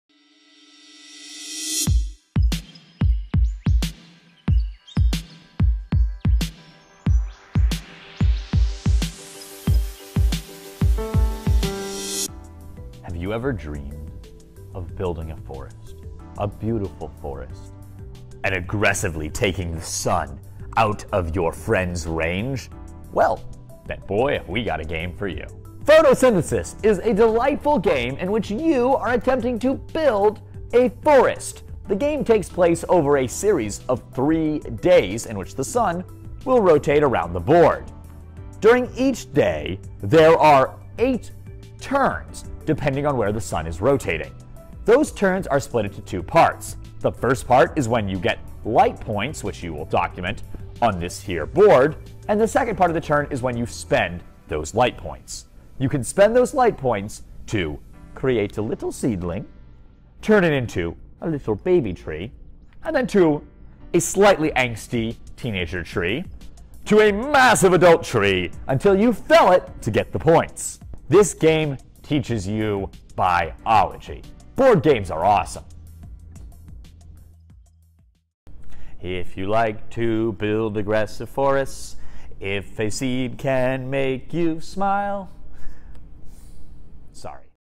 Game of the Week: Photosynthesis
Do your aspirations involve aggressively growing a forest to hog all the sunlight to yourself? If so - we've got a game for you!